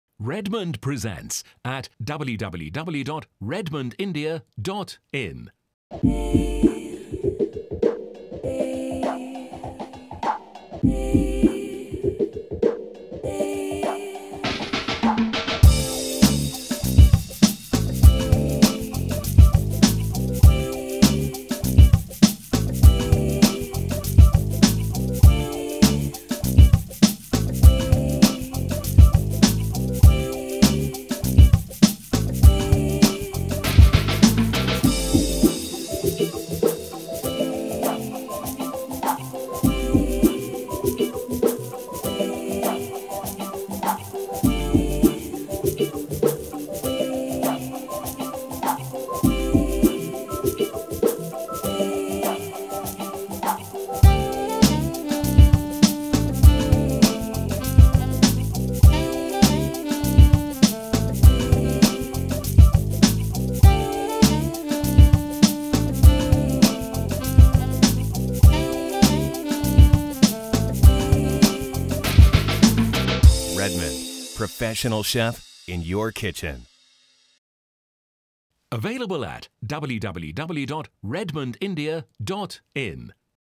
Hot sandwiches in REDMOND RAG-241 All-purpose multipurpose Halogen oven
Wonderful brown pastry, perfectly done grilled meat, tender fish, poultry with astoundingly even golden brown crust - everything is cooked without oil! Is it possible? With the multipurpose air grill REDMOND RAG-241-E you may forget about extra calories and cholesterol. Convection thermal food processing technology realized in it allows to achieve wonderful results: hot air stream, circulating in working chamber, renders fat from any product (meat, fish, poultry), so that the ready dish is soft and rich. One more advantage of the air grill REDMOND RAG-241-E is its small size. The air grill is combined with a transparent glass lid. Thanks to 22.5 cm diameter you may use any suitable heat-resistant dishware as a working capacity. Besides, REDMOND RAG-241-E will be perfectly suitable for cooking food in multicookers of any brands with 4, 5 and 6 litre capacity. Cook deliciously! Eat healthy with the multipurpose REDMOND air grill!